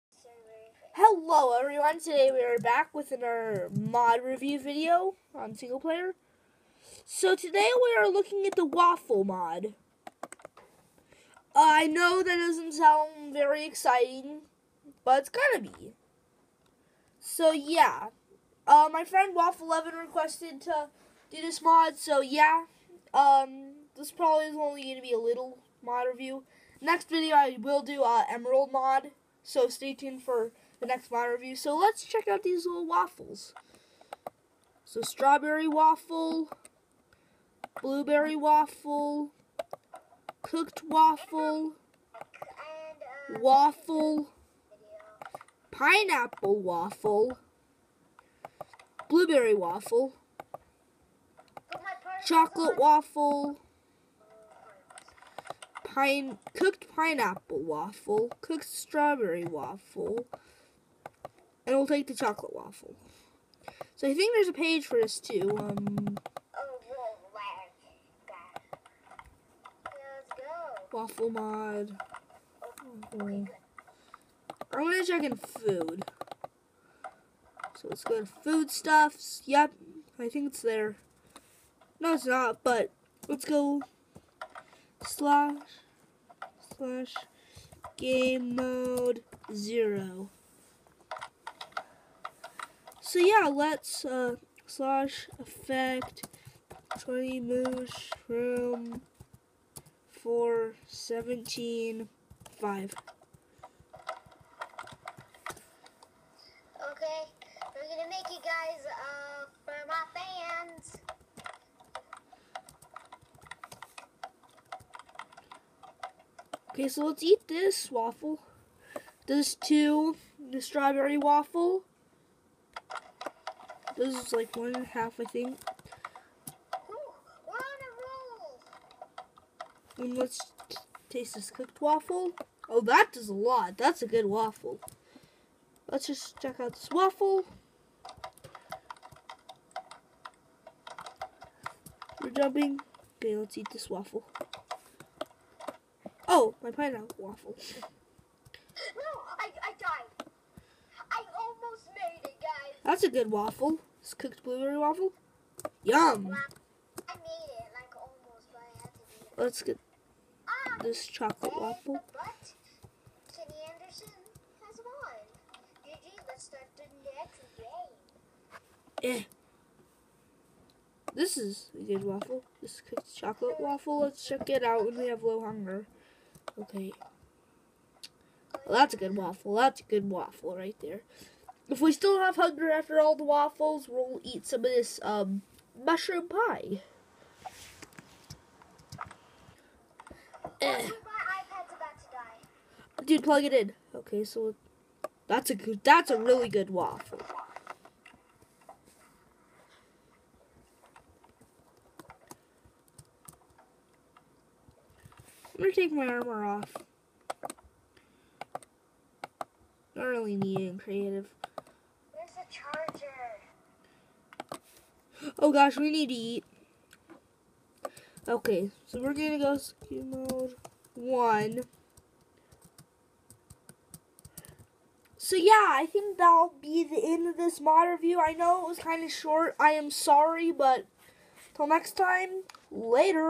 Waffle mod review
Hi guys today we review the waffle mod. It means a lot thx!